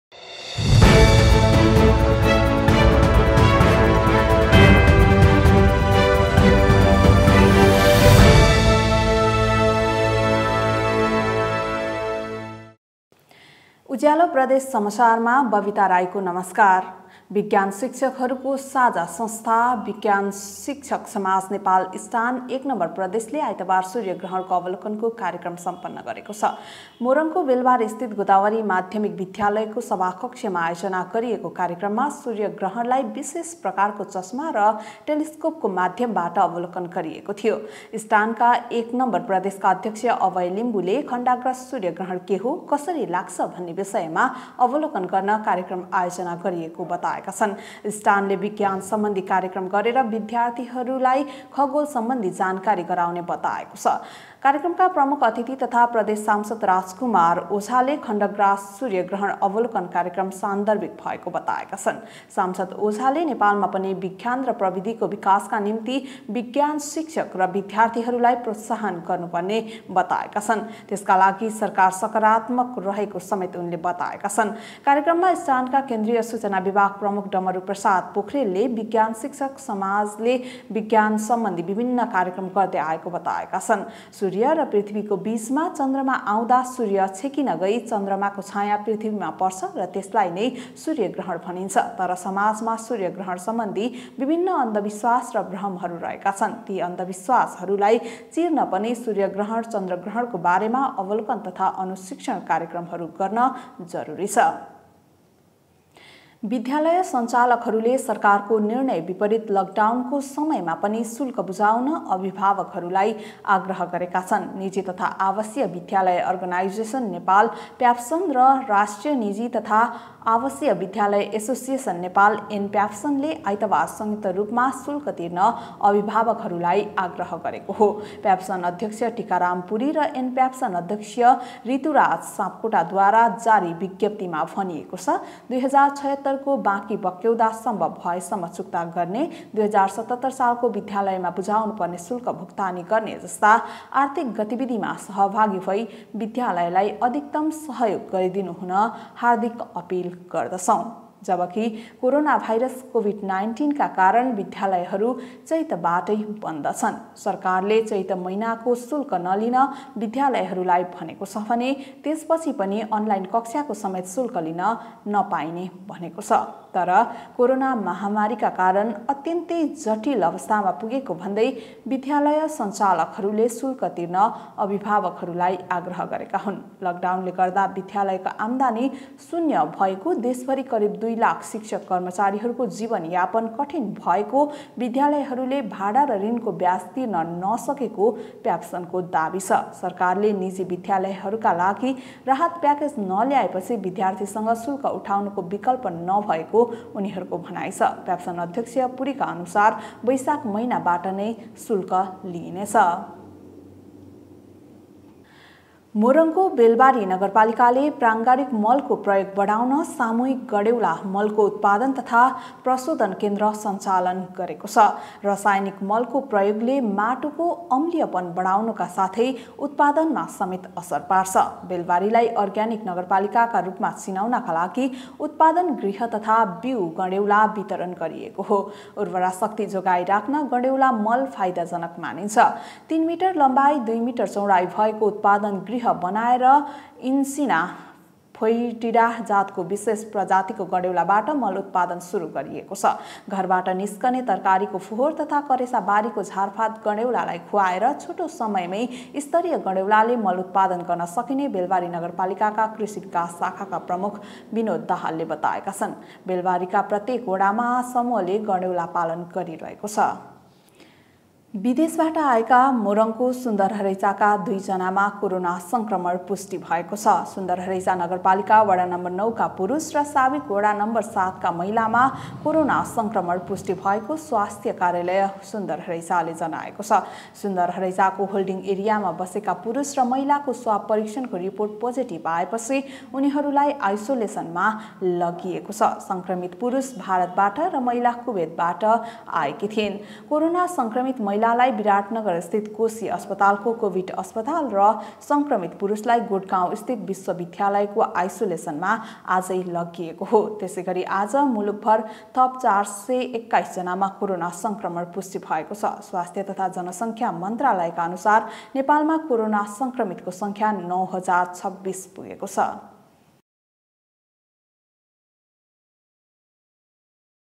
उज्यालो प्रदेश समाचार । असार ७ २०७७
बेलबारीमा सूर्य ग्रहणको अवलोकन । निजी विद्यालयले चैत देखिकै शुल्क लिने ।। सुन्दरहरैंचामा थप दुईजनामा कोरोना पुष्टि  ।।। उज्यालो प्रदेश समाचार । असार ७ २०७७